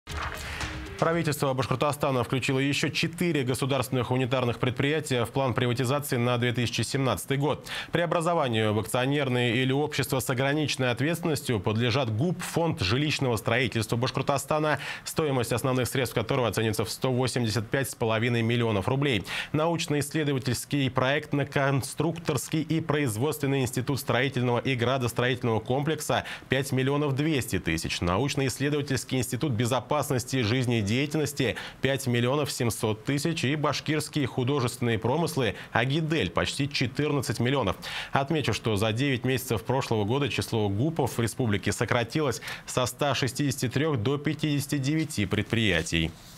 Еще 4 госпредприятия РБ включены в план приватизации ГУПов на 2017 год
Правительство Башкортостана включило еще четыре государственных унитарных предприятия в план приватизации на 2017 год. Преобразованию в акционерные или общества с ограниченной ответственностью подлежат ГУП Фонд жилищного строительства Башкортостана.